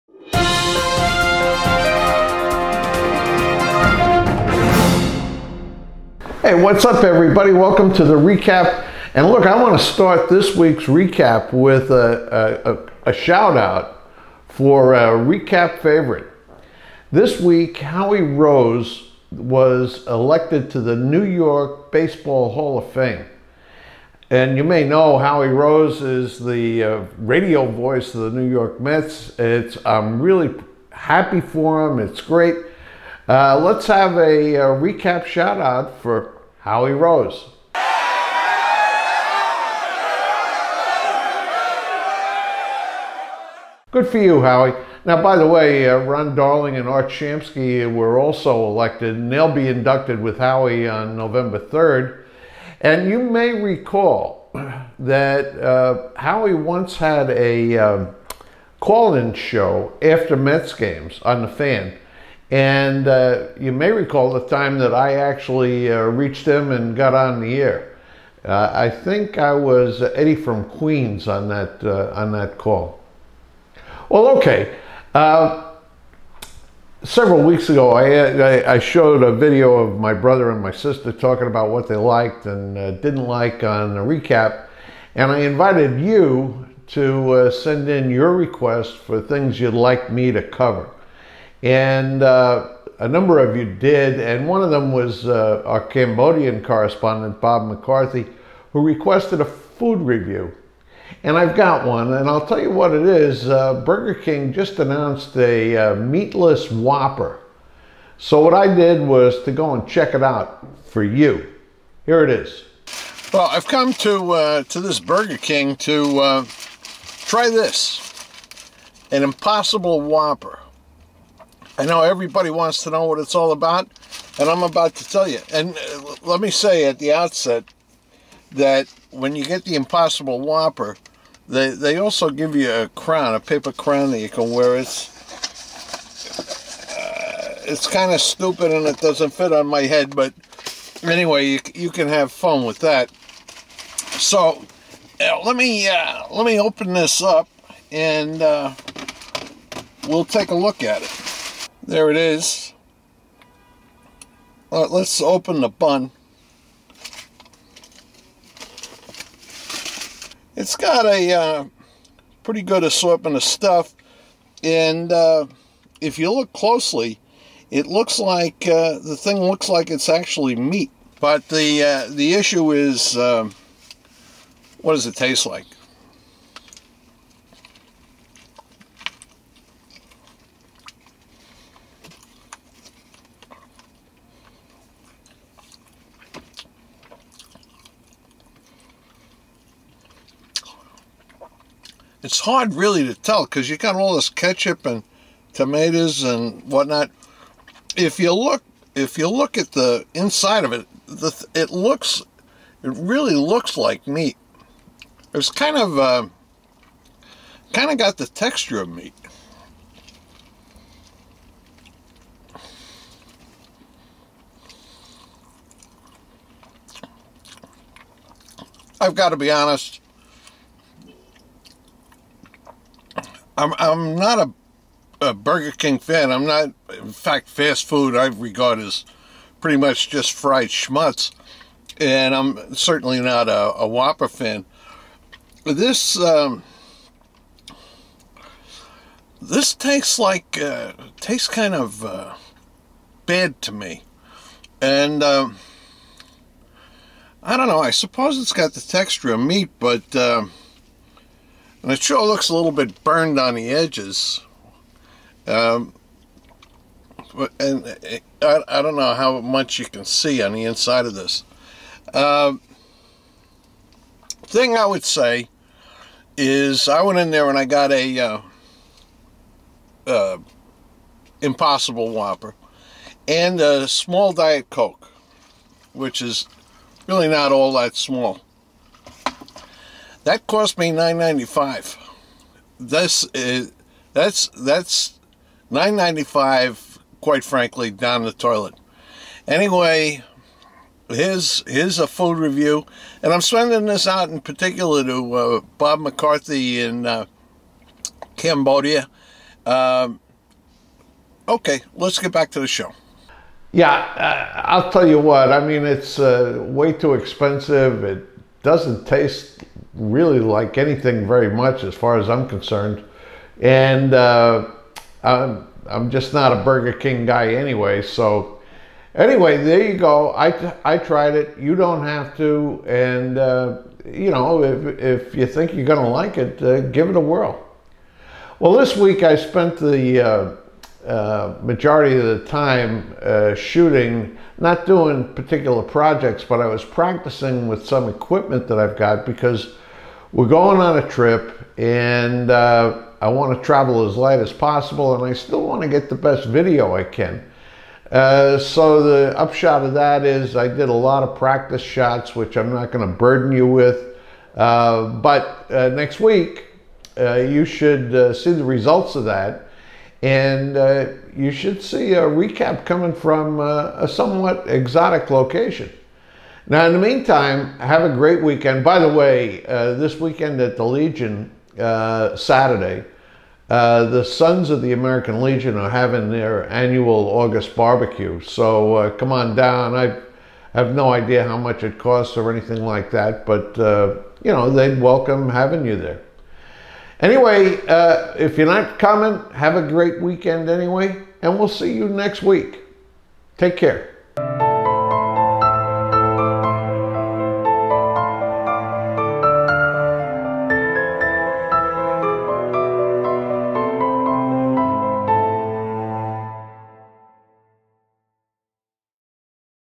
The Weekly Recap - August 16, 2019
An in-depth review of Burger King's Impossible Whopper. Plus an update on recent activity